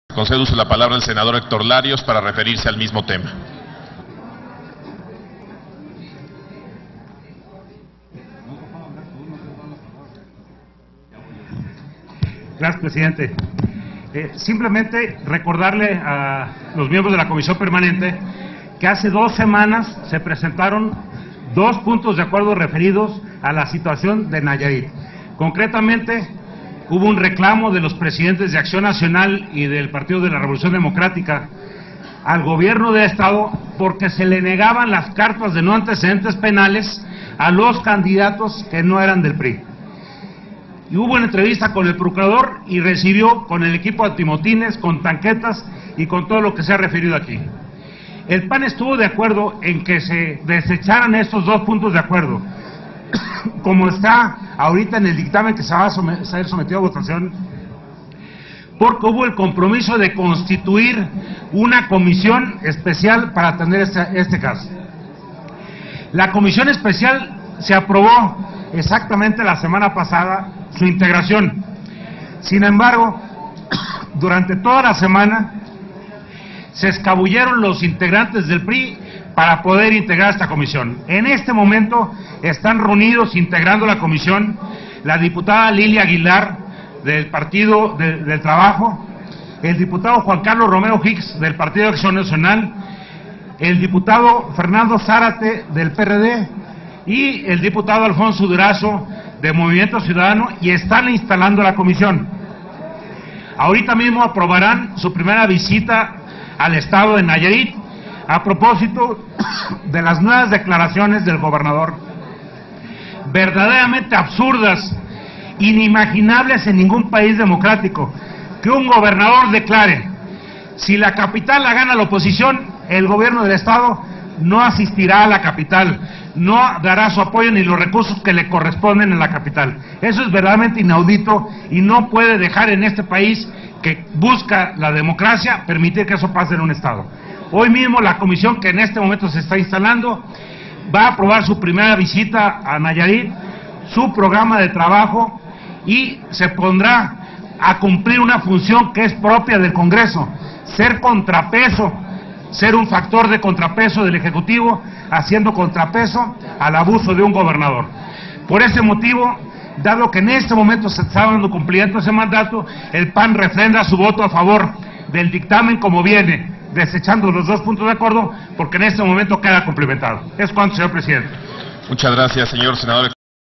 Senador Héctor Larios habla dictamen acerca de elecciones en Nayarit
El Sen. Héctor Larios señaló que es inaudito que un gobernador declare que no dará su apoyo en caso de que gane la oposición.